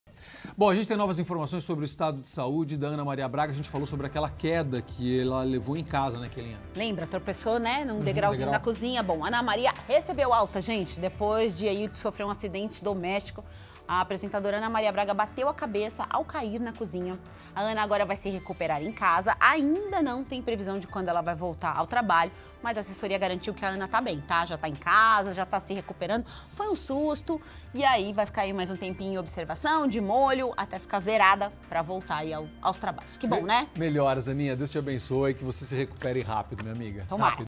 Ana Maria Braga recebe alta do hospital
A apresentadora Ana Maria Braga recebeu alta do hospital depois de sofrer um acidente em casa. A apresentadora bateu a cabeça ao cair na cozinha e agora vai se recuperar em casa. Ainda não tem previsão de quando Ana irá voltar ao trabalho.